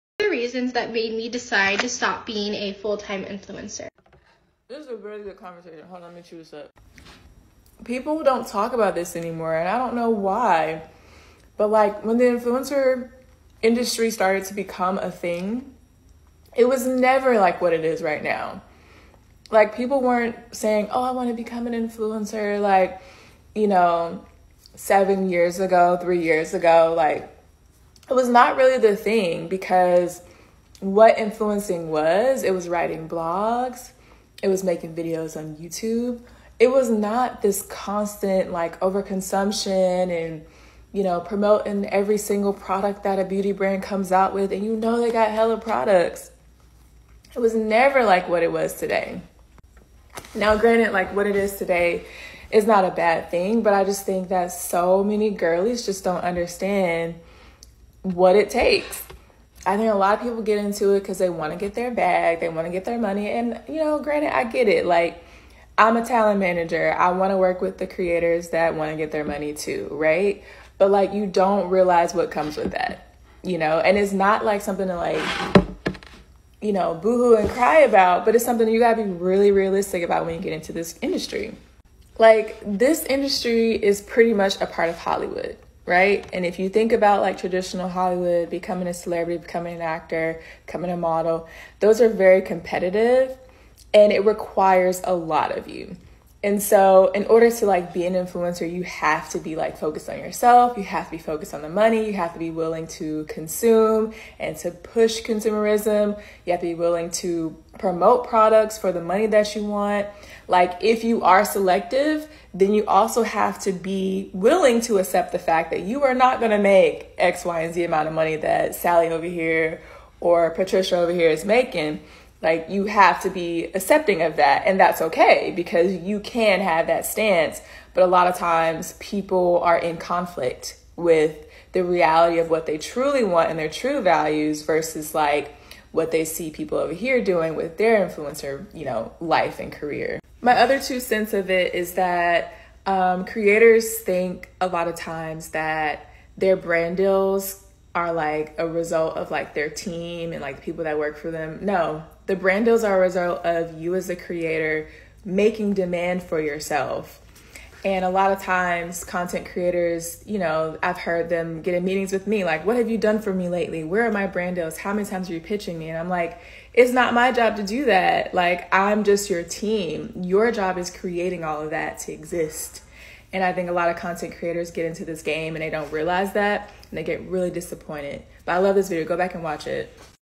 BEFORE becoming an INFLUENCER see this!
UNSOLICITED ADVICE: Really think about and consider what you stand for as a creator. It's a sea of creator's out there. How will you stand out?